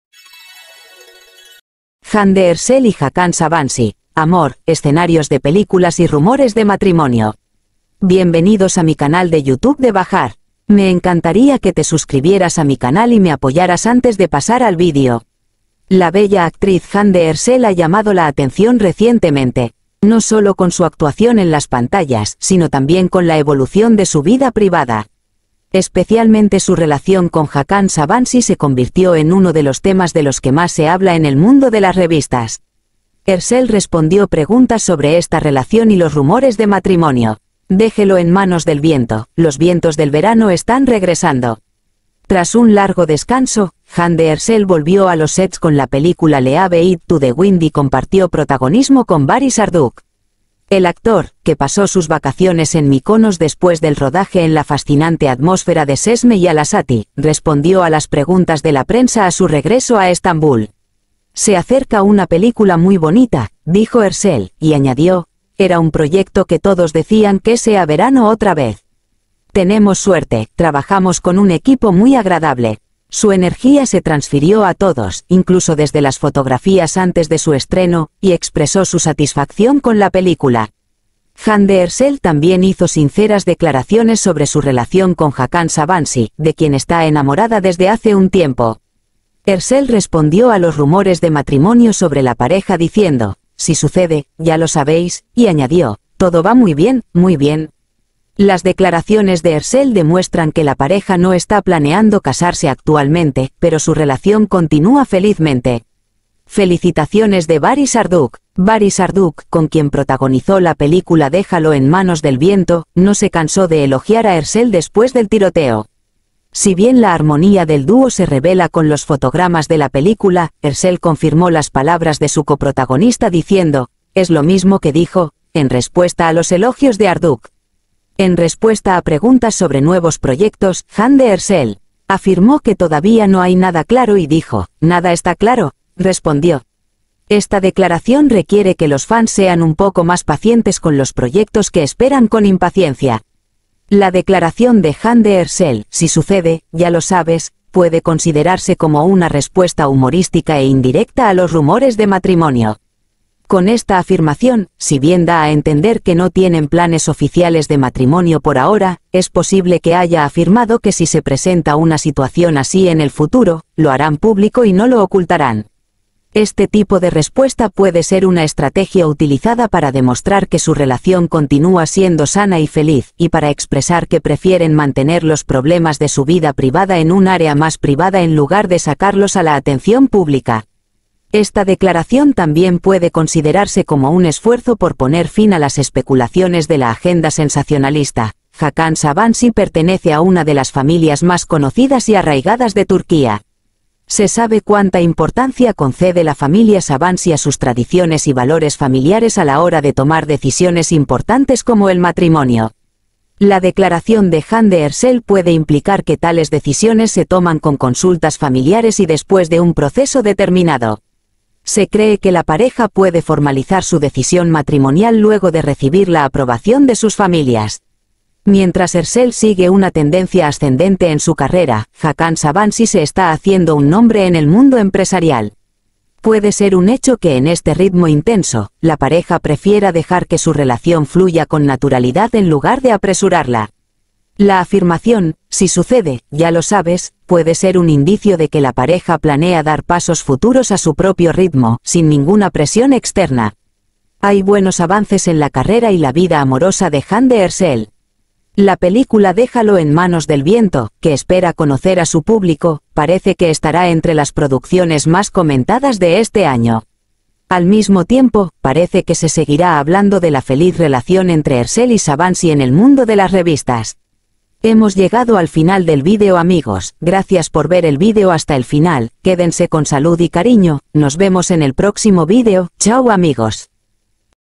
La respuesta de Hande Ercel a los rumores de matrimonio: "Si sucede, ya lo sabes"
Hande Ercel regresó a los sets con su nueva película 'Leave it to the Wind' y compartió protagonismo con Baris  Arduc La bella actriz, que pasó sus vacaciones en Mykonos después del rodaje en la fascinante atmósfera de Çeşme y Alaçatı, respondió a las preguntas de la prensa a su regreso a Estambul. "Se acerca una película muy bonita", dijo Erçel, y agregó: "Fue un trabajo en el que todos dijeron 'que sea verano otra vez'. Tenemos suerte, trabajamos con un muy buen equipo. Su energía se transfirió a todos, incluso desde las fotografías antes de su estreno", dijo, expresando su satisfacción con la película.
Hande Ercel también hizo sinceras declaraciones sobre su relación con Hakan Sabancı, de quien está enamorada desde hace un tiempo. Erçel respondió a los rumores de matrimonio sobre la pareja diciendo: "Si sucede, ya lo sabéis" y añadió: "Todo va muy bien, muy bien". Las declaraciones de Erçel demuestran que la pareja no está planeando casarse actualmente, pero su relación continúa felizmente.